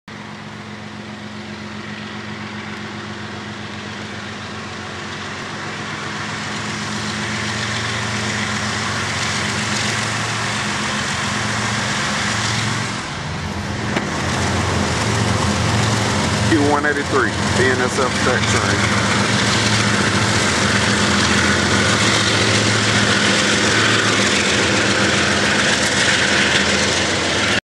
CSX Q183, passing By Z-Q LACATG Bham AL 5-15-111
CSX Q183, BNSF Z Train to ATL Z LACATG, Wanted to chase it, but by the time I got to where I was headed he was already moving quick and ahead of me.